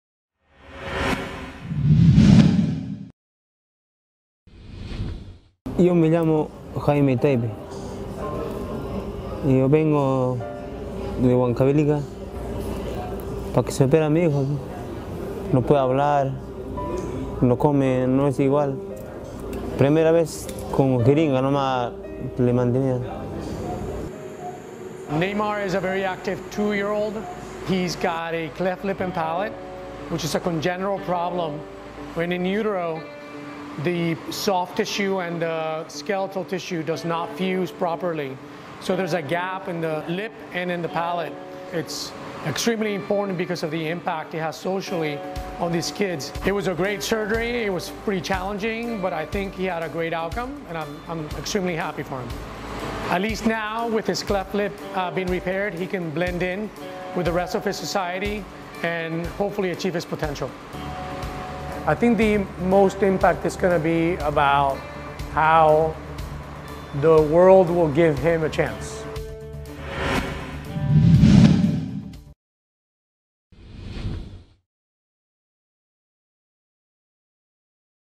Medical Team Aboard USNS Comfort Perform Cleft Palette Surgery
A Venezuelan father talks about his son's operation for a cleft palette aboard the hospital ship USNS Comfort (T-AH 20). Comfort is working with health and government partners in Central America, South America, and the Caribbean to provide care on the ship and at land-based medical sites, helping to relieve pressure on national medical systems strained by an increase in Venezuelan migrants. (U.S. Navy video by Mass Communication Specialist 3rd Class Maria Llanos)